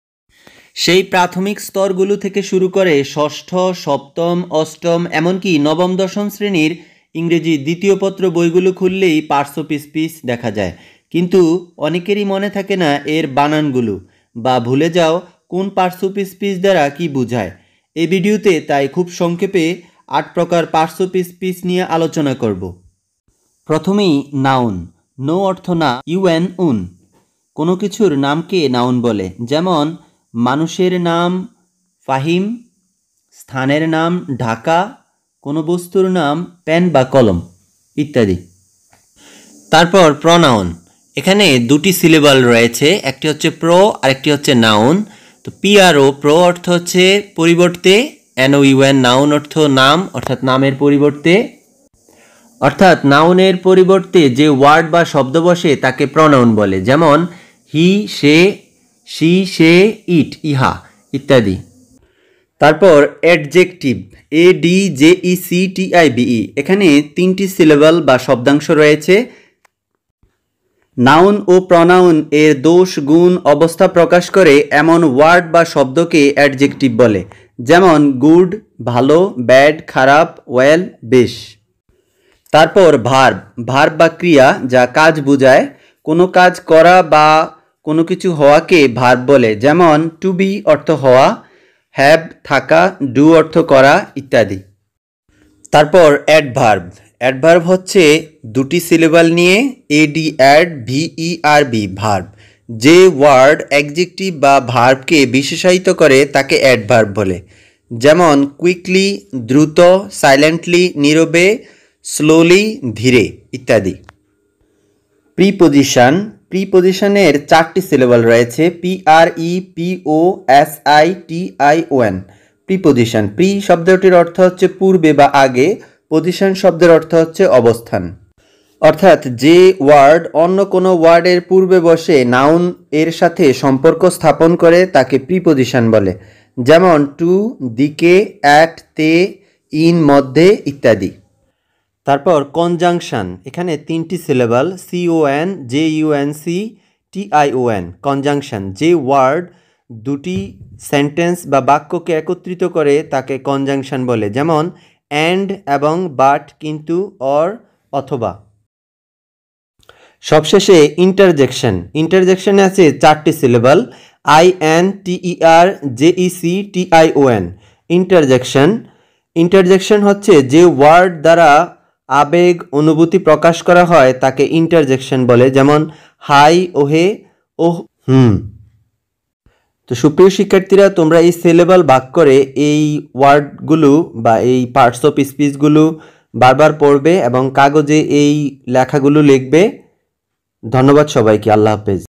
Parts of Speech শিখে নাও খুব সহজে
মুখস্ত হবে তোমারও

NOUN -  A noun is the name of a person, place, thing, or idea.
- pen, dog, work, music, town, London, teacher,

PRO_NOUN - A pronoun is a word used in place of a noun.

AD_JEC_TIVE - An adjective modifies or describes a noun or pronoun.
 - good, big, red, well, interesting

VERB - A verb expresses action or being.
(to) be, have, do,

AD_VERB - (Adverb describes a verb, adjective or adverb)
- quickly, silently, badly.
মনে রাখার কৌশল (word এর শেষে ly থাকে)

PRE_PO_SI_TION - (Preposition links a noun to another word)
to, at, in

CON_JUNC_TION - Conjunction joins clauses or sentences or words
And, But, Or

IN_TER_JEC_TION - (Short exclamation, sometimes inserted into a sentence)
Oh!, Hmm!, Hi!

🌸 English Grammar - Number, Person, Gender & Parts of Speech all in one vedio for the young learners 🌸